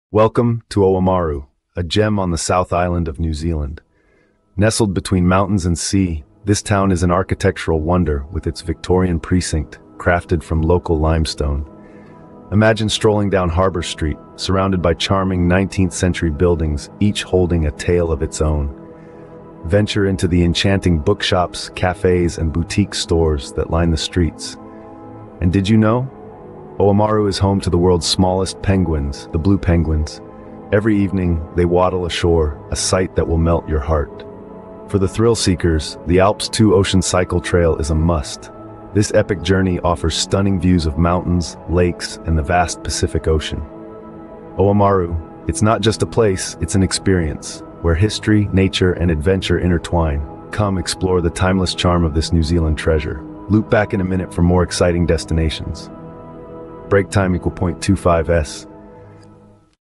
Oamaru, South Island, New Zealand
In this captivating video, we invite you to join us on a virtual journey to Oamaru, a hidden gem nestled in New Zealand's stunning South Island. Prepare to be awe-struck as we showcase the unparalleled beauty and irresistible charm of this captivating town.

Immerse yourself in picturesque landscapes as we explore the breathtaking coastline, quaint streets, and historic architecture that defines Oamaru. Discover the unique attractions that make this town truly special, from the enchanting Victorian Precinct to the fascinating Steampunk HQ.

Join us as we delve into Oamaru's rich cultural scene, where art and creativity flourish in every corner. Get a glimpse into the vibrant local art galleries, bustling artisan markets, and the thriving arts and crafts community that adds an extra layer of allure to this hidden gem.

Uncover the secrets of Oamaru's fascinating past as we visit heritage sites and learn about the town's important role in New Zealand's history. Experience the nostalgia of a bygone era as we showcase the preserved Victorian architecture that transports you back in time.

Whether you're a travel enthusiast looking for your next adventure or simply a curious soul seeking the lesser-known wonders of the world, this video is not to be missed. Sit back, relax, and let the beauty of Oamaru unfold before your eyes.

Don't forget to leave a comment sharing your thoughts and experiences. Get ready to be captivated by Oamaru's irresistible charm!